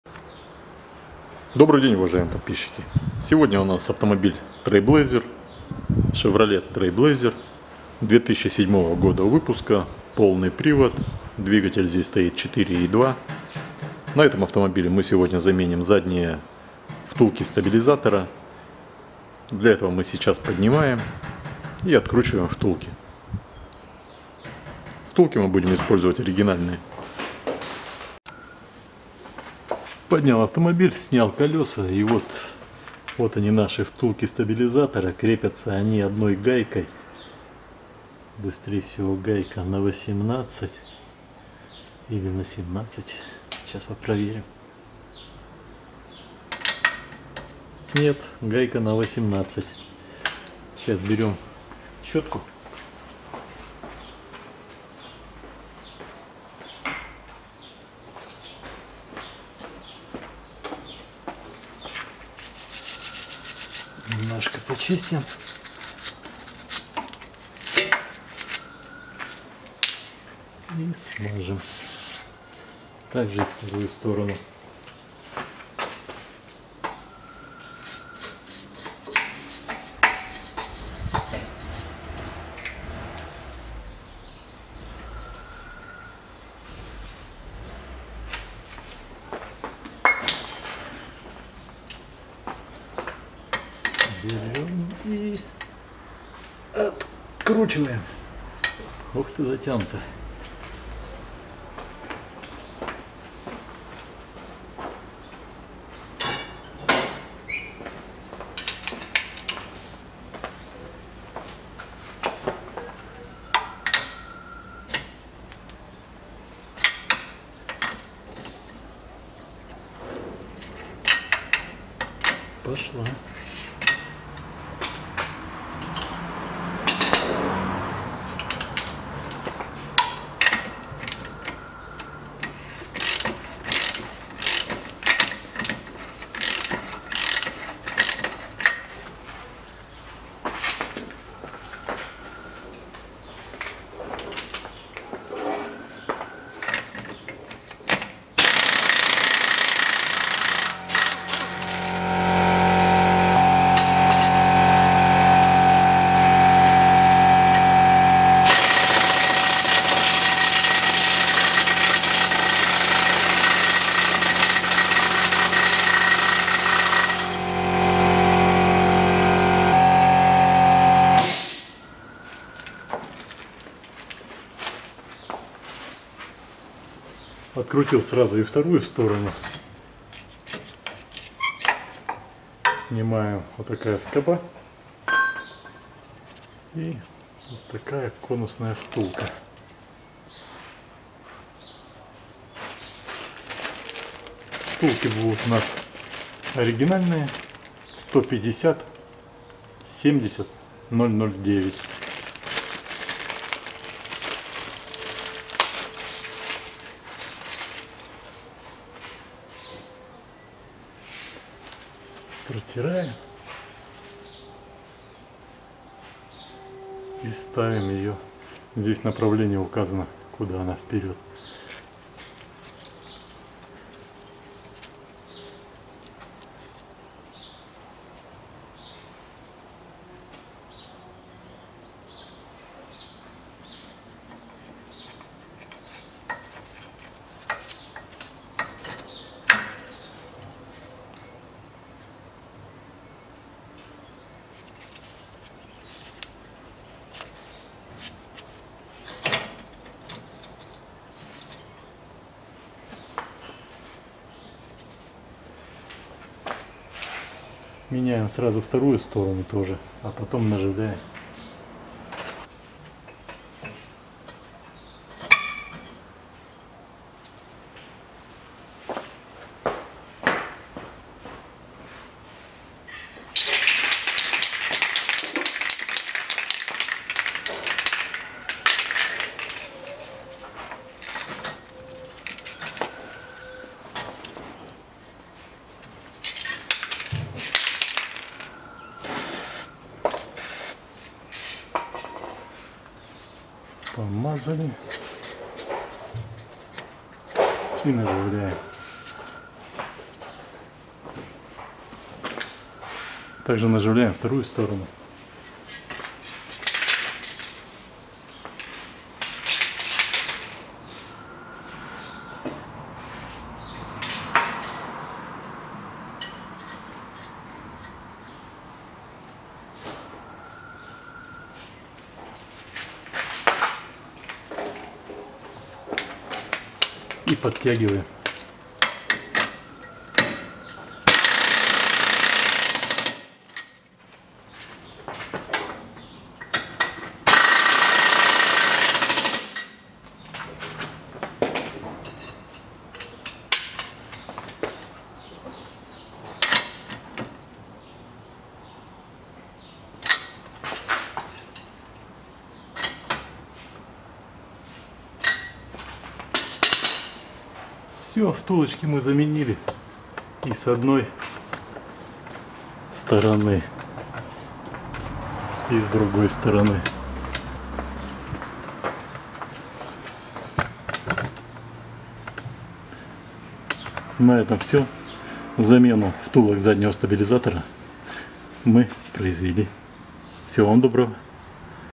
Chevrolet TrailBlazer Шевроле Трейлблейзер 4,2 2007 года Замена втулок стабилизатора заднего мост
Запчасти для своих нужд заказываю в Краснодаре в магазине TWIN-CAM по т. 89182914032

втулка стабилизатора  GM  15070009